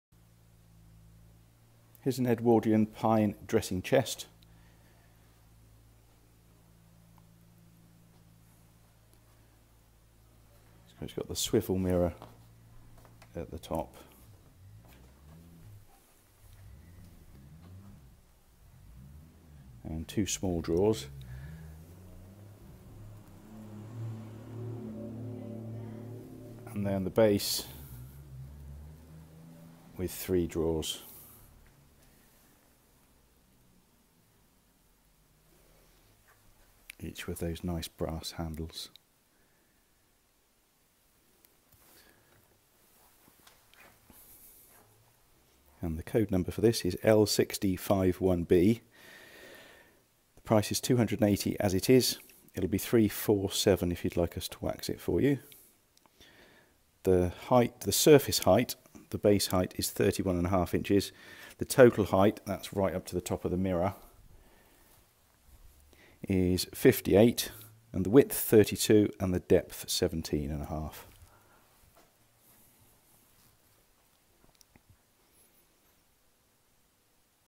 Edwardian Pine Dressing Chest - Pinefinders Old Pine Furniture Warehouse
Here for sale is a lovely Edwardian pine dressing chest in excellent condition for its age.  A swivel mirror and two small drawers to the top section and three full width drawers to the base. Each drawer with the original brass drop handles.

Code number L6051B
Price as it is in the bare wood £280. Price if you would like us to wax it for you £347

Height 58" (31.5" surface height), width 32", depth 17.5"

I'm afraid this has now been sold. All of our country pine dressing chests we still have for sale can be seen on our website here: